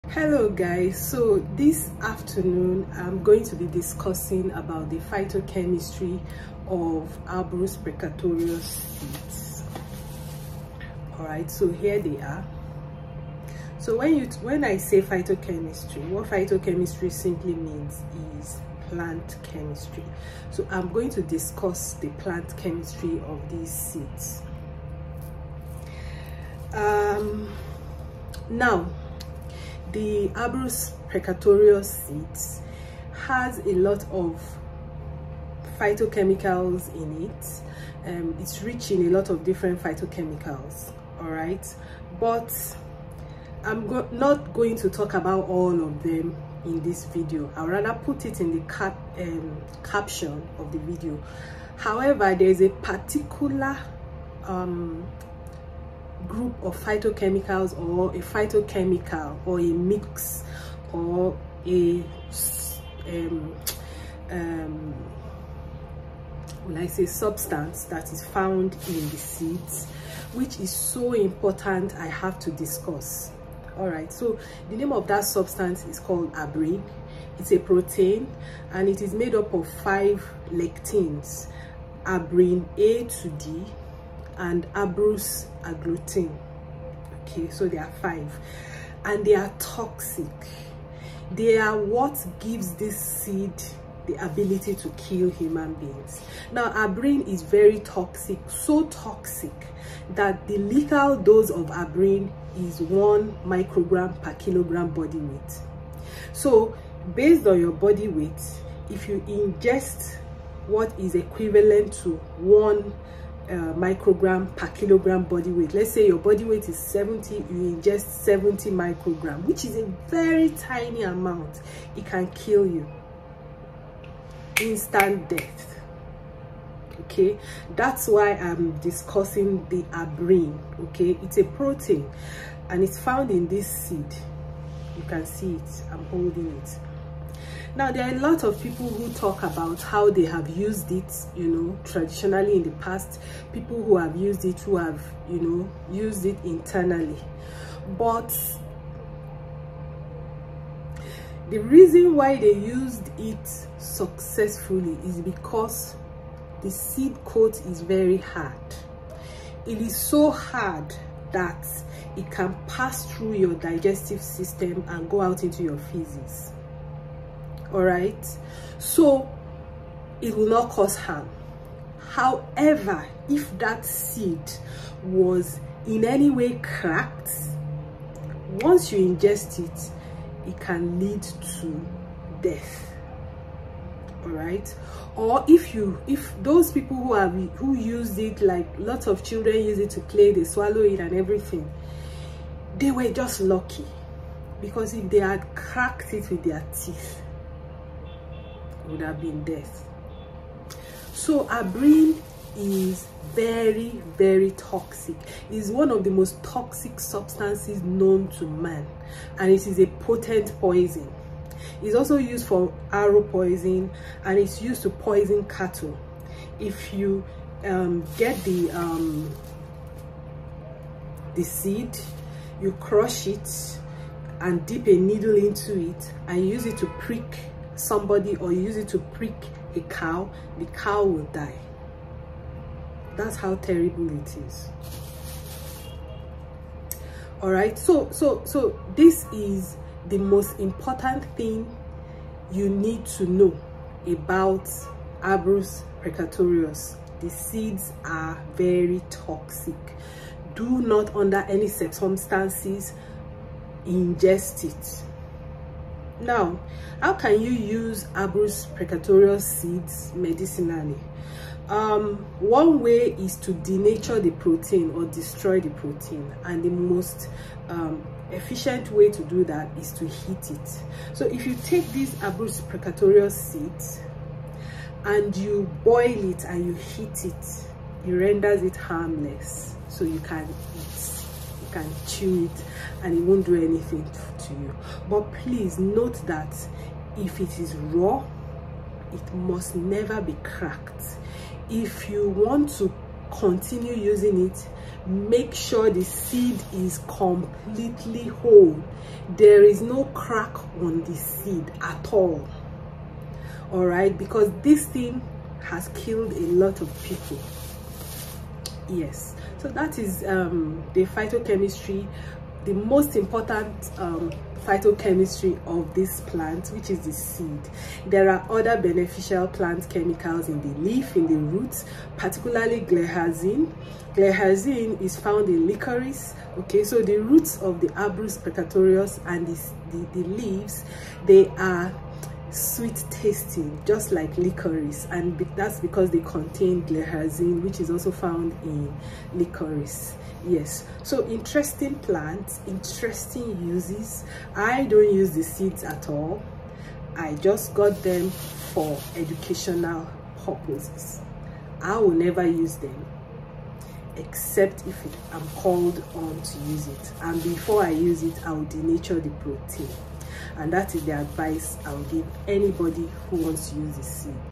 Be careful with this poisonous herb!
Phytochemistry of Abrus precatorious

As most plants are, Abrus precatorious is rich in plant chemicals
Its leaves contain traces of abrin, abruslactone A, triteepernoids abrusoside A-D hemiphloin, abrusoside, sugars, glycyrrhizin, as well as numerous other plant secondary metabolites

Its roots also contain glycyrrhizin, some proteins, anthocyanins, alkaloids and flavonoids

Glycyrrhizin is mainly found in licorice and is the phytochemical that gives it the sweet taste, it’s interesting to note that Abrus precatorious leaves and roots are another source of glycyrrhizin

The seeds though are really the most important plant part because of its content of abrin, a toxic mix of lectins. As a explained in the video abrin a very potent poison, so potent one seed can kill an adult. Please watch the video in full to understand how lethal it is

Under no circumstances should the seeds be ingested. If you must ingest it, the toxic abrin must be denatured as I explained in the video